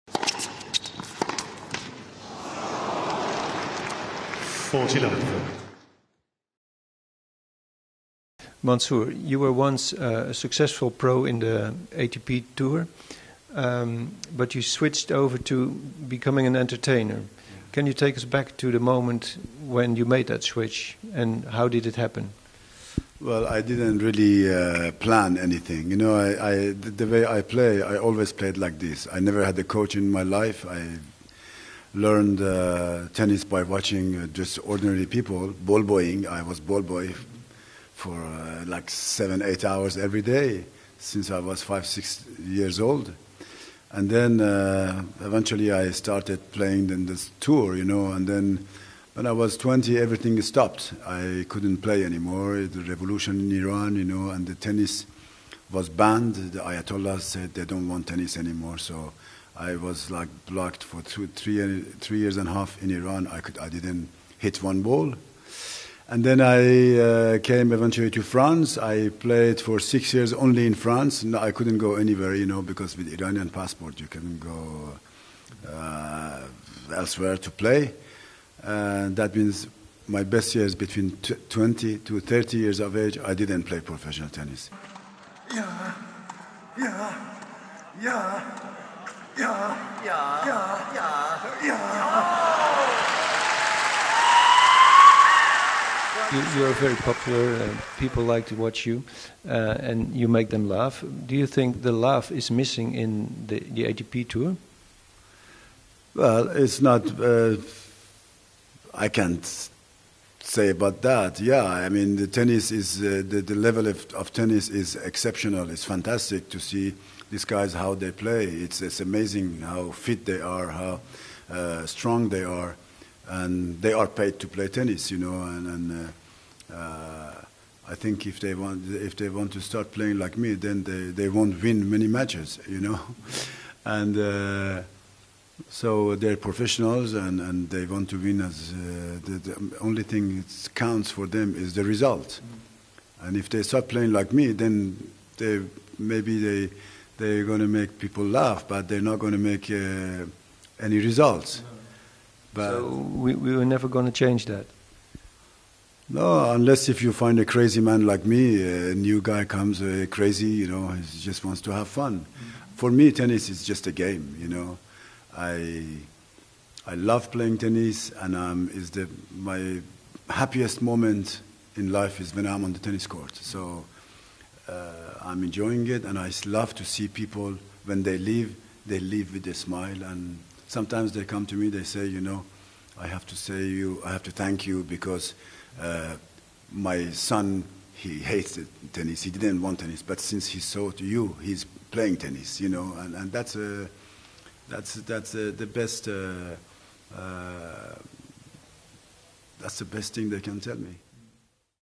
Bahrami: geen coach om trucjes af te leren
Deze Tennisbalgoochelaar was ook weer van de partij tijdens de AFAS tennisclassics 2012